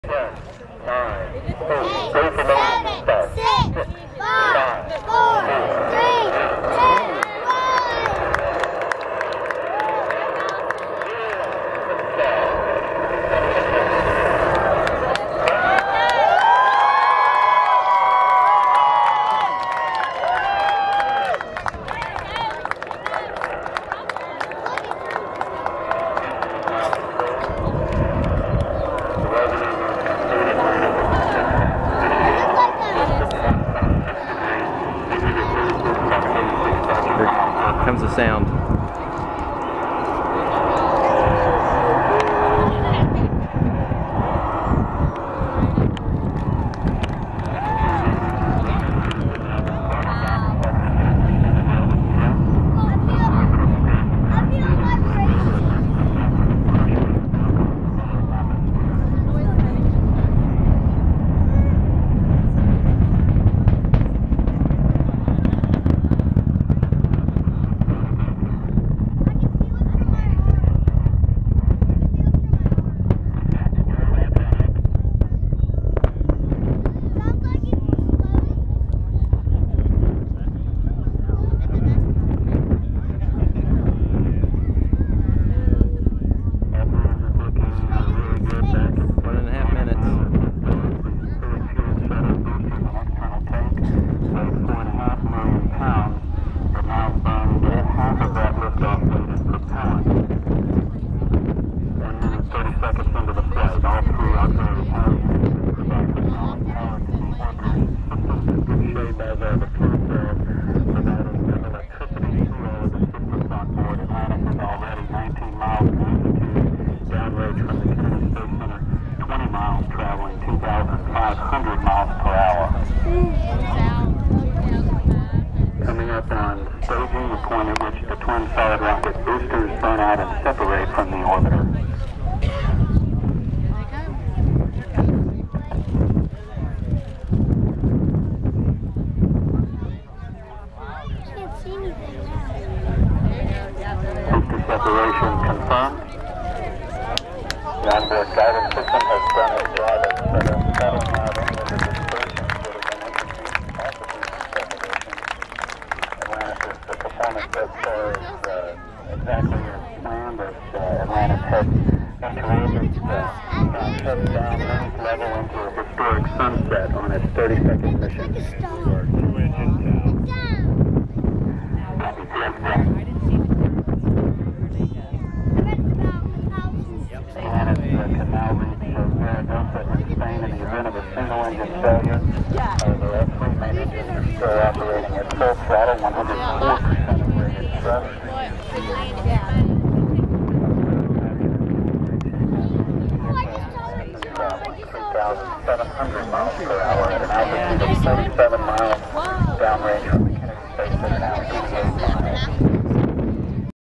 Space Shuttle STS 132 Atlantis launch 05 14 2010
Space Shuttle STS-132 launch on May 14 2010.  Near the end of the video you can see the booster rocket separation (you have to look close! - at 2 min 23 sec into the video).  This is taken from the NASA Causeway at Kennedy Space Center.